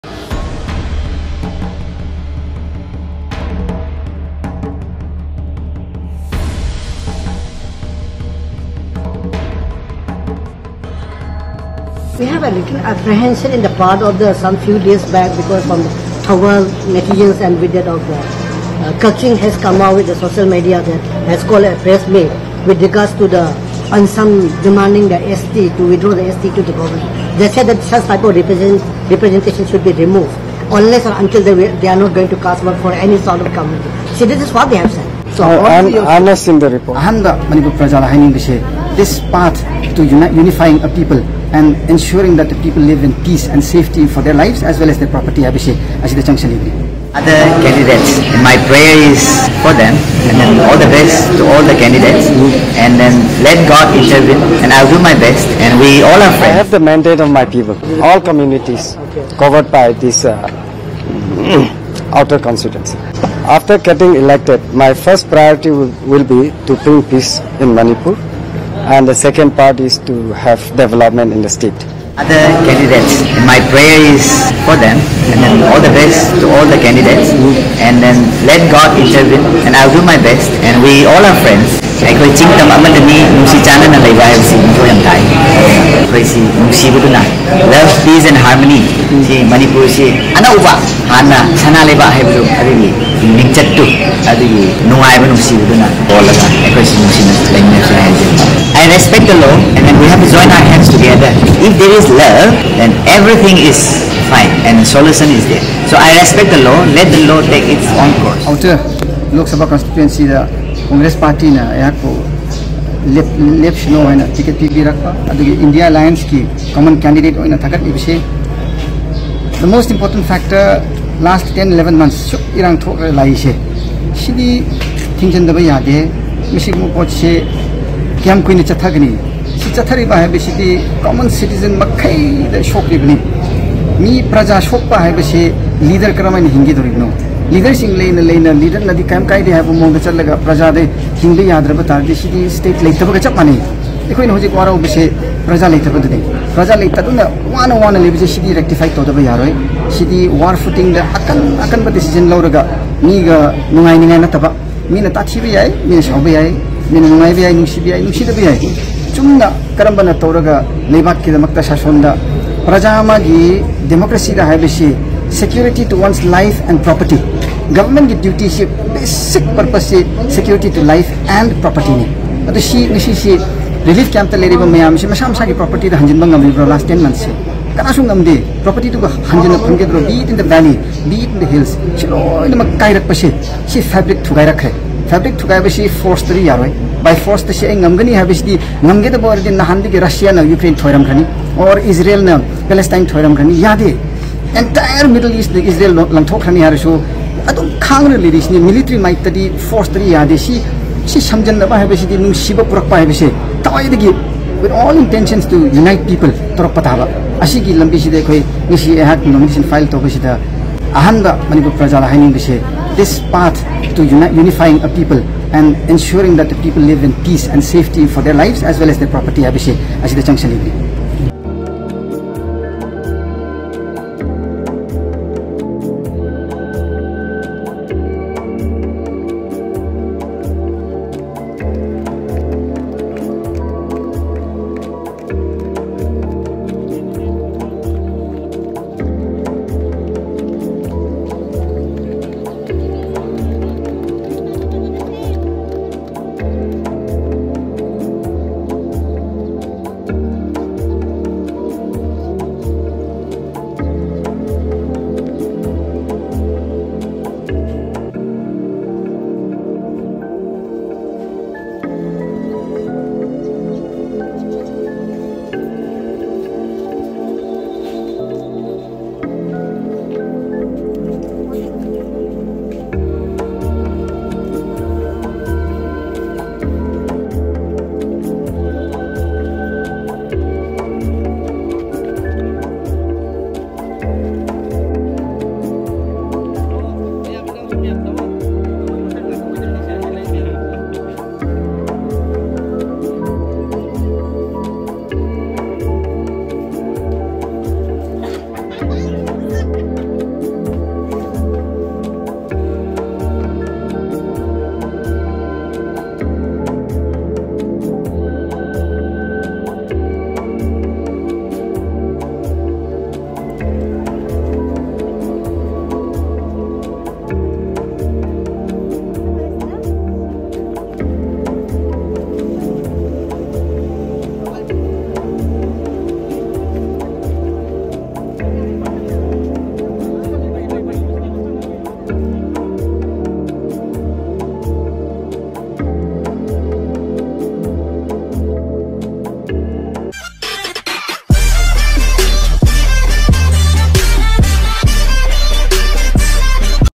18th Loksabha Election 2024.
Outer Manipur Parliamentary Election da Mirep Oigadouriba Alfred Kanngam Arthur( INC) candidate with INDIA Bloc,Kachui Timothy Zimik(NPF) Dr.Alysion Abonmai (Independent).